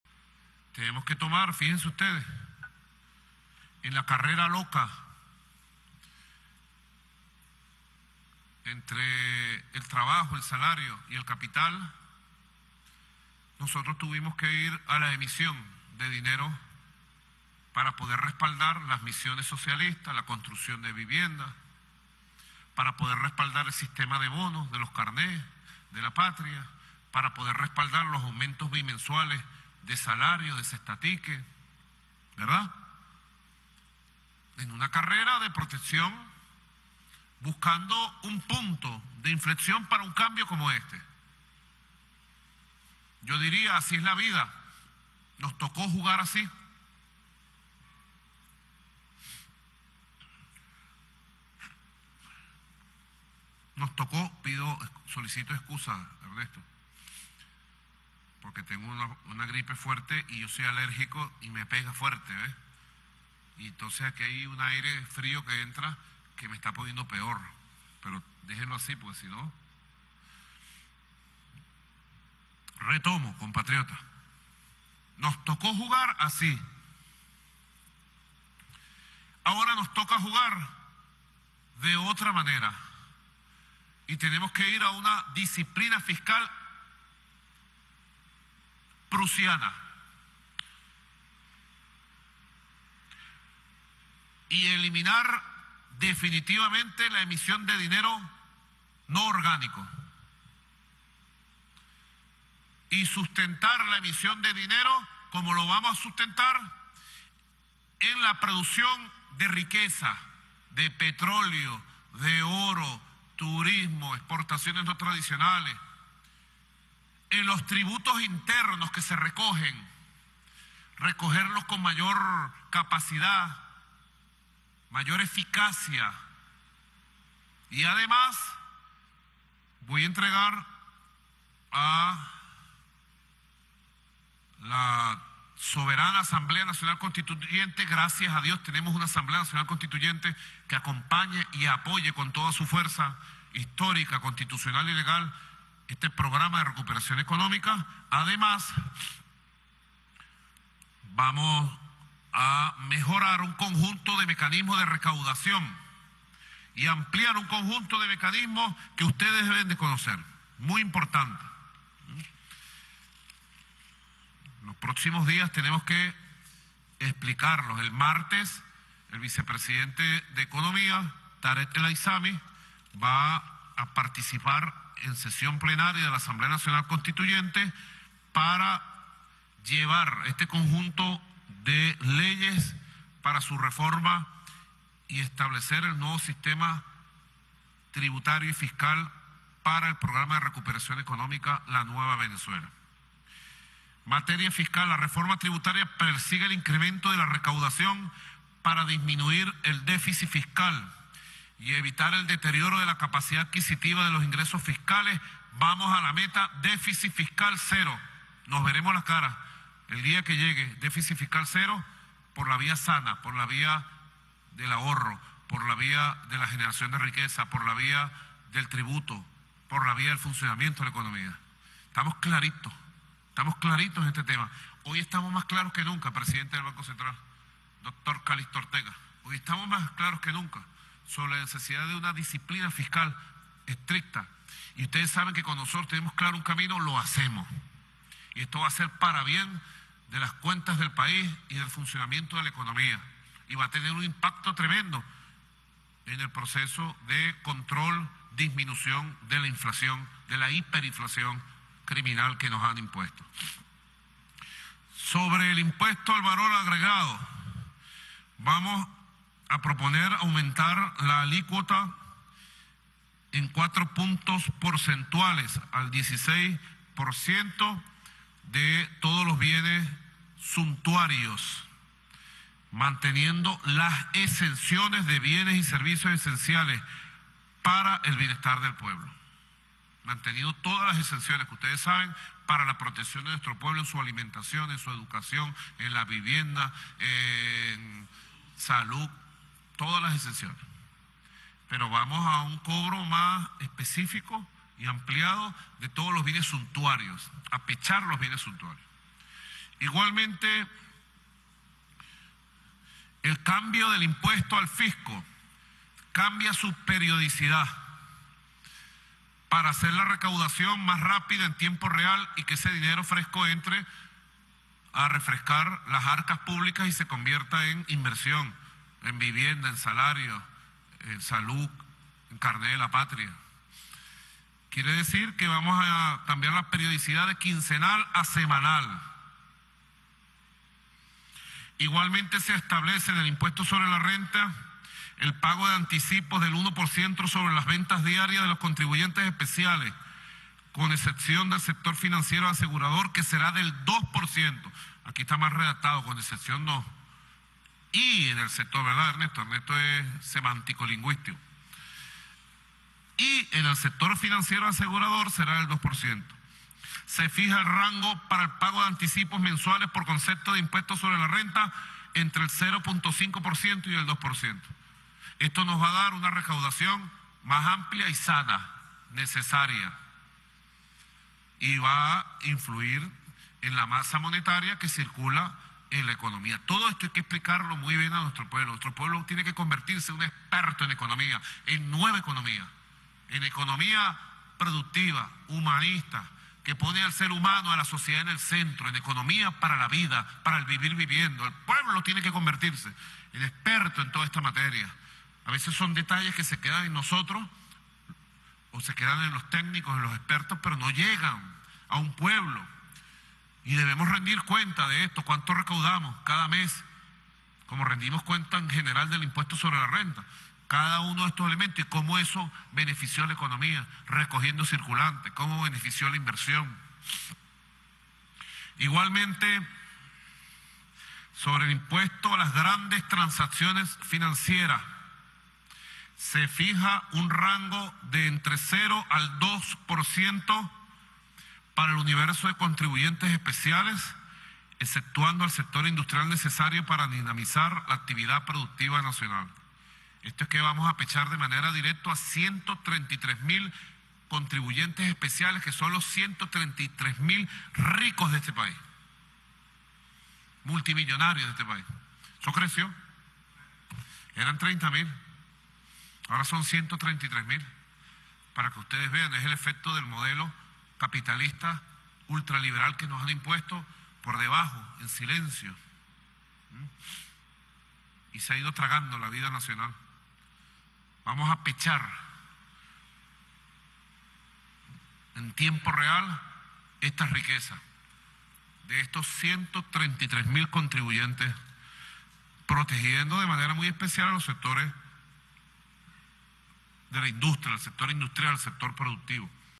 Maduro anuncia medidas de disciplina fiscal y aumenta IVA de 12 a 16 por ciento
Como parte del plan de  recuperación económica, el Presidente venezolano Nicolás Maduro anunció este 17 de agosto de 2018 diferentes medidas de disciplina fiscal, entre ellas el subir el IVA de 12 a 16 por ciento y el anunciar diferentes medidas para recaudar impuestos de las grandes empresas.